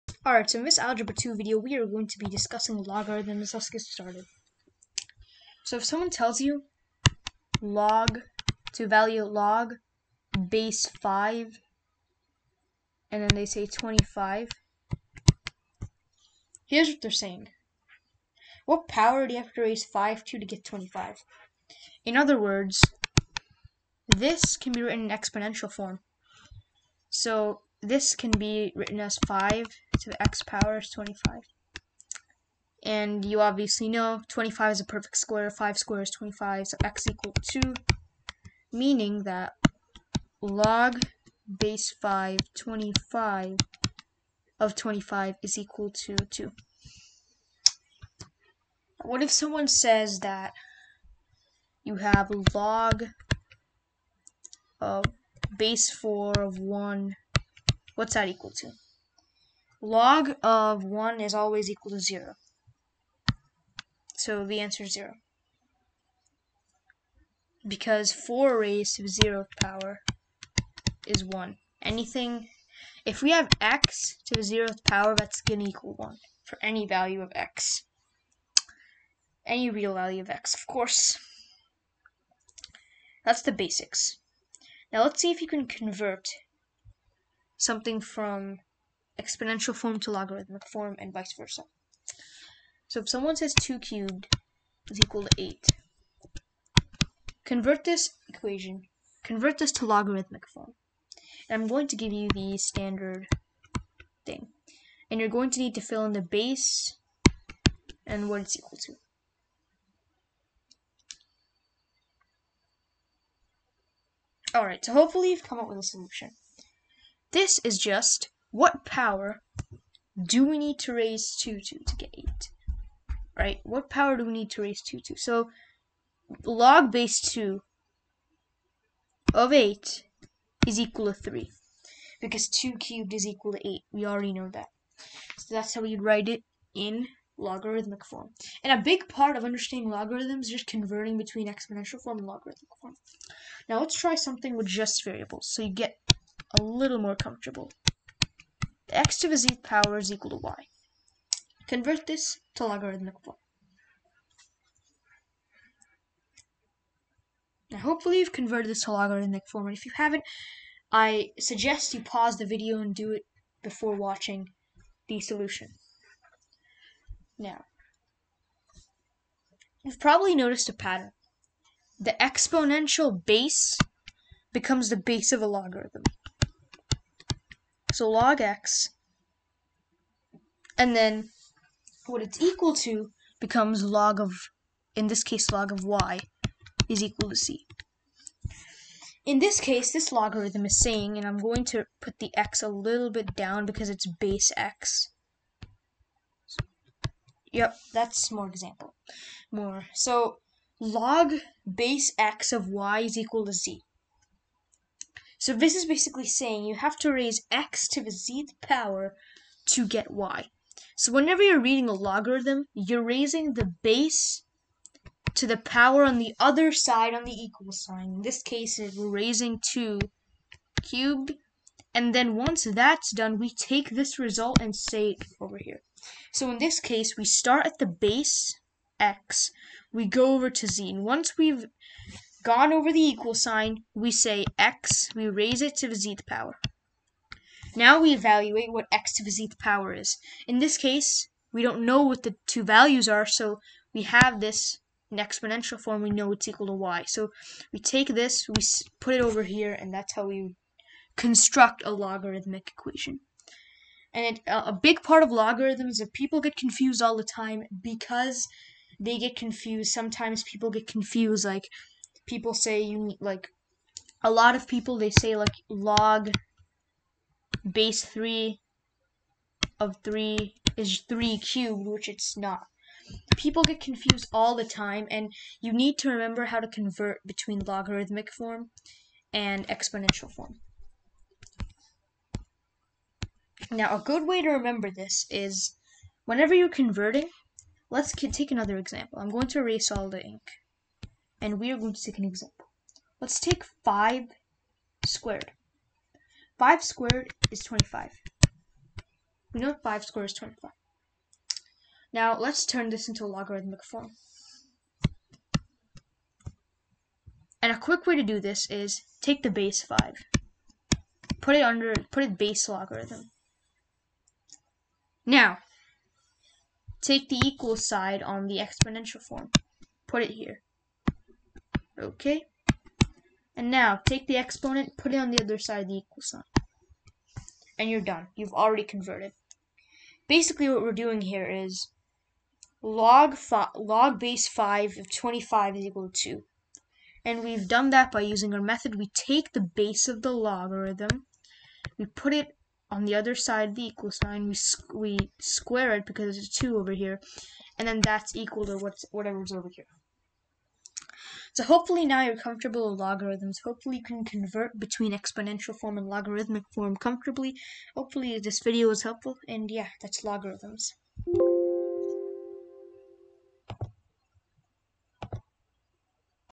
Introduction to Logarithms
This Algebra 2 video introduces the basics of logarithms.